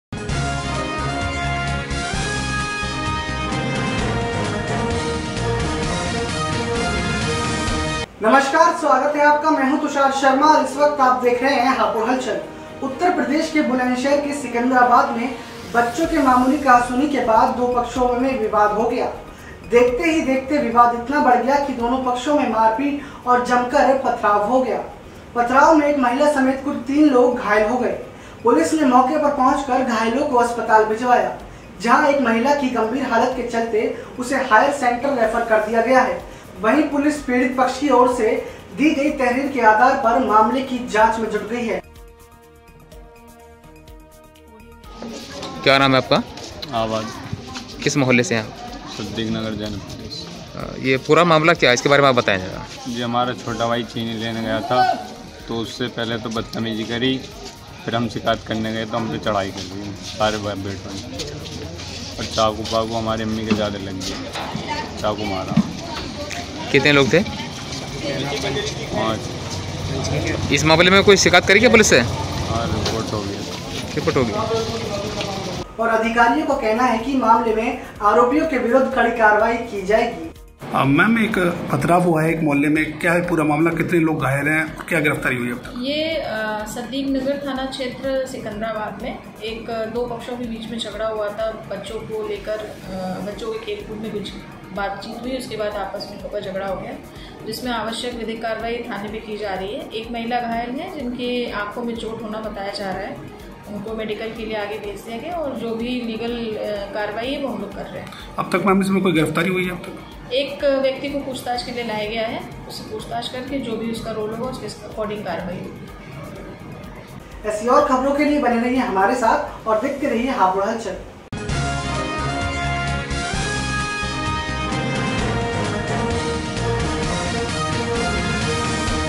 जनपद में मामूली विवाद में पथराव
बच्चों के मामूली विवाद में पथराव, बड़े आए बीच में हुई मारपीट-पथराव।

मामले में एक पक्ष से महिला समेत तीन घायल

पुलिस ने मौके पर पहुंच घायलों को अस्पताल भिजवाया

गम्भीर हालत में महिला हायर सेंटर रेफर

पीडित पक्ष की ओर से पुलिस को दी तहरीर

सिकंदराबाद के मोहल्ला सद्दीकनगर का है मामला।